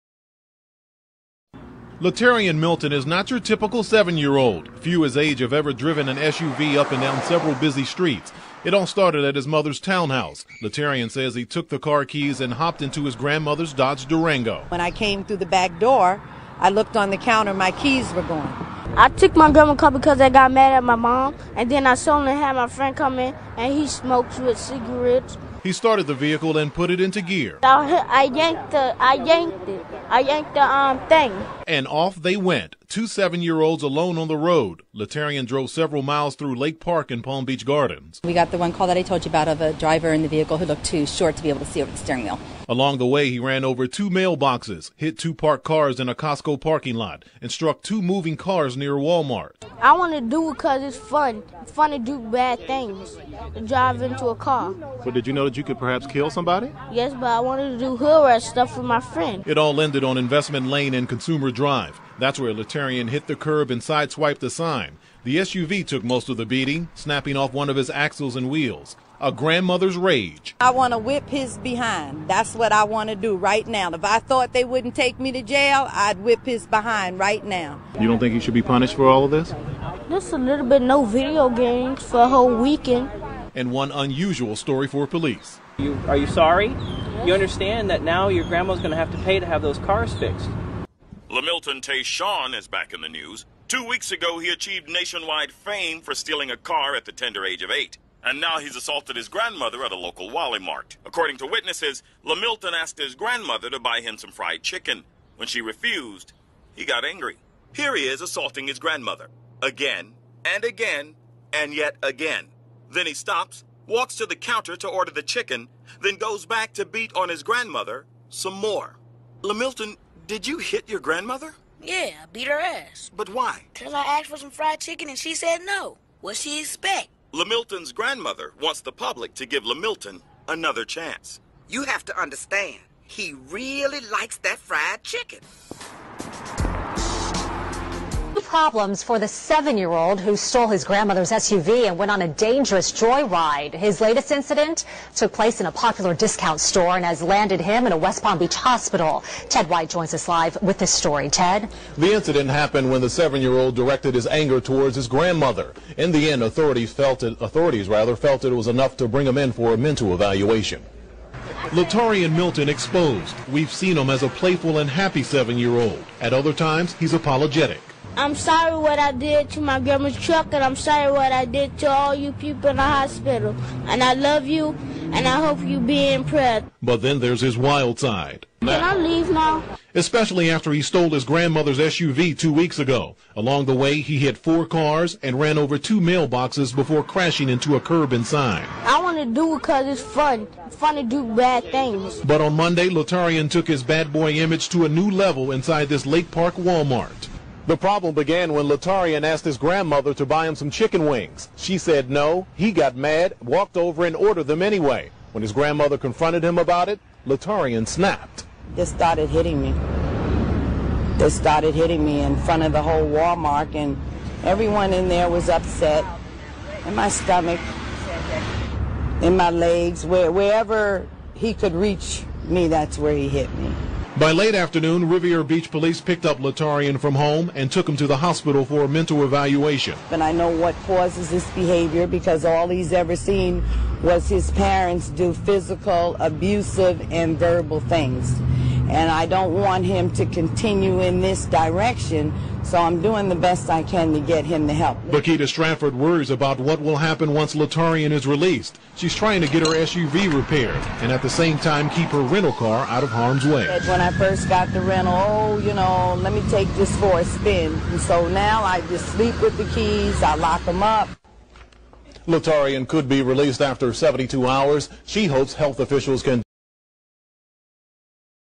The "Real" Boondocks: Lamilton smoking with cigarettes.
The inspiration behind some of The Boondocks' funniest episodes.